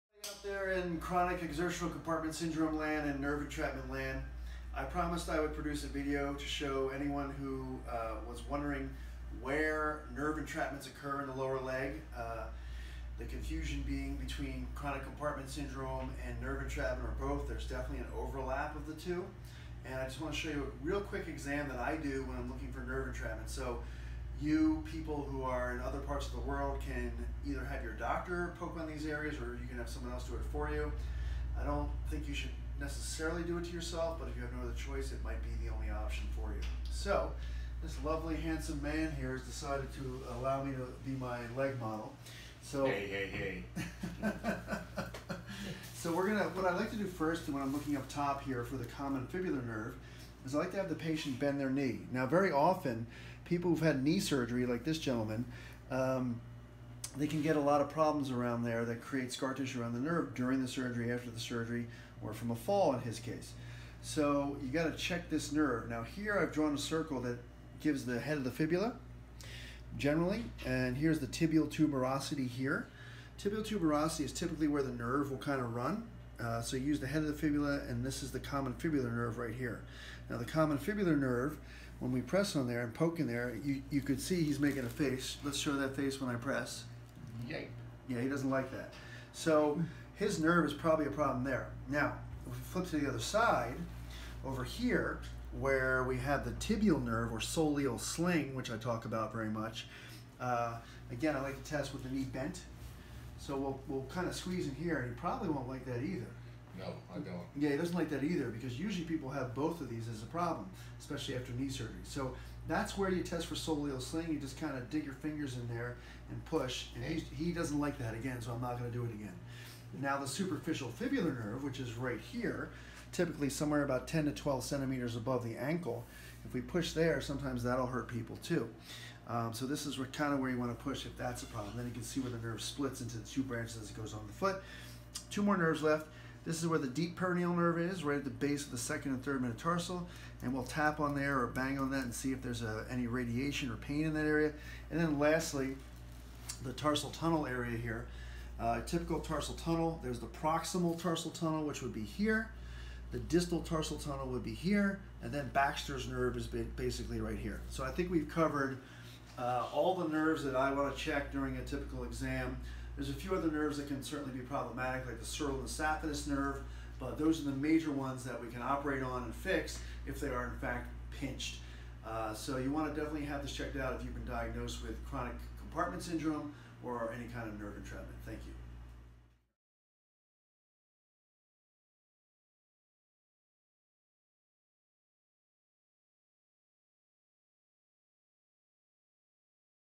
Nerve Entrapment Exam ( CECS crossover )
This is a very quick exam for nerve entrapment and this can be seen in people diagnosed with CECS there can be an overlap of the two diagnoses.  Most doctors do not look for the nerve entrapments and rely on pressure testing or EMGs which are not always accurate.